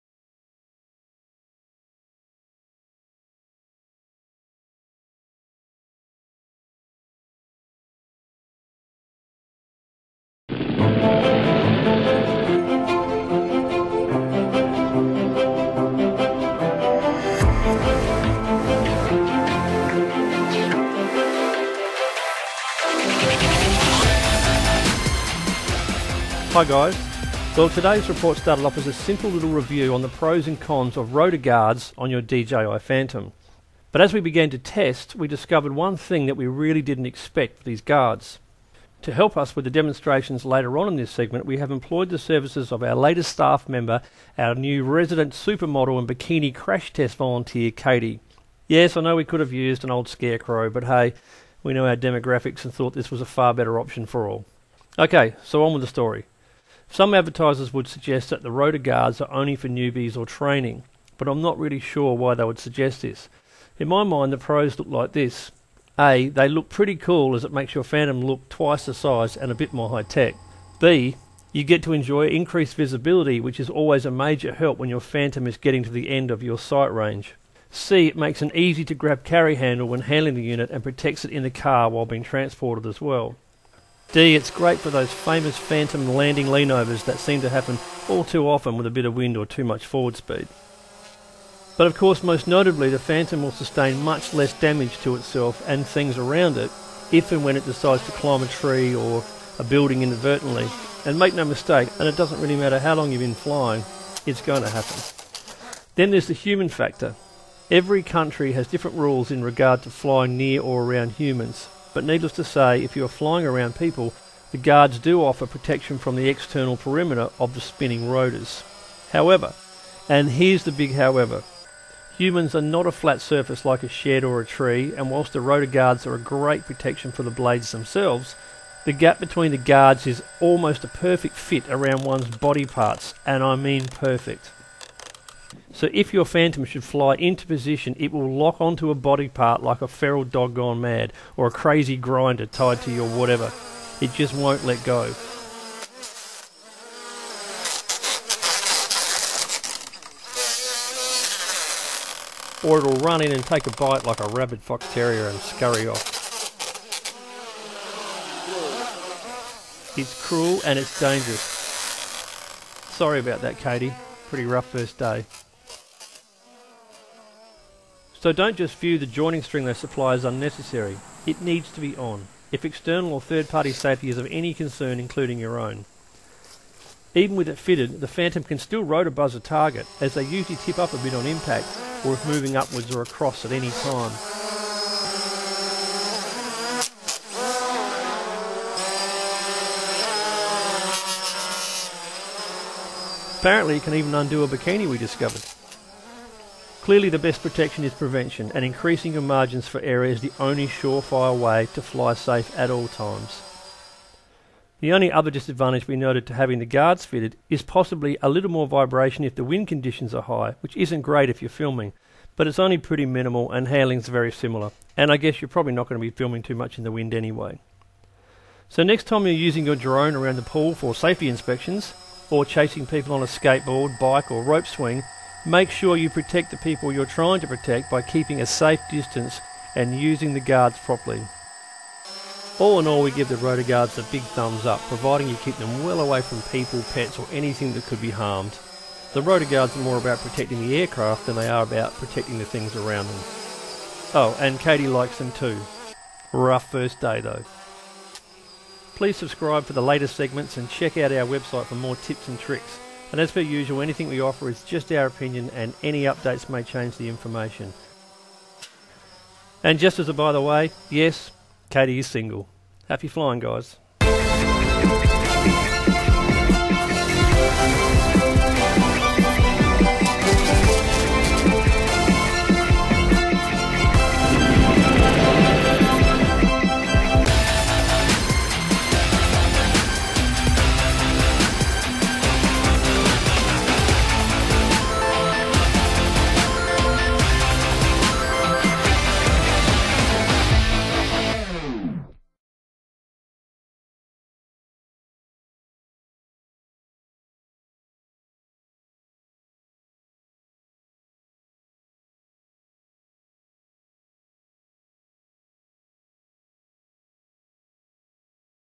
Rotor Safety Guards. DJI Phantom drone crashes into test crash bikini girl & undoes her top!?
What happens when a DJI Phantom drone hits a crash test bikini girl super model or any human for that matter!? How good are Propeller / Rotor Guards for the DJI Phantom?
To buy or not to buy rotor guards? That is the question!
What happens when a phantom hits a human?

for tips & tricks

If you were actually just looking for BOOBS & DRONES, check out this video

The best prices & deals we've found for DJI Phantom equipment

A simple little review on the pros and cons of propeller (rotor) guards on your DJI Phantom

But as our tests went on, we discovered one thing that we really didn't expect!

We introduce our new resident super model bikini crash test volunteer, Katie! Yes I know we could have just used an old scarecrow, but hey we know our demographics and thought this was a far better option!

In my mind, the pros look like this!

A: They look pretty cool as it makes your Phantom look twice the size and a bit more high tech!!

B: You get to enjoy Increased visibility, which is always a major help when your phantom is  getting to the end of your sight range

C: It makes an easy to grab carry handle when handling the unit and protects it in the car while being transported as well

D: There's more!!

And of course most importantly ... you need to watch the video for the key points and the bikini crash!

DJI Phantom, Phantom 2, Phantom Vision

Filmed on a GoPro and Panasonic Lumix Camera

for our list of upcoming tips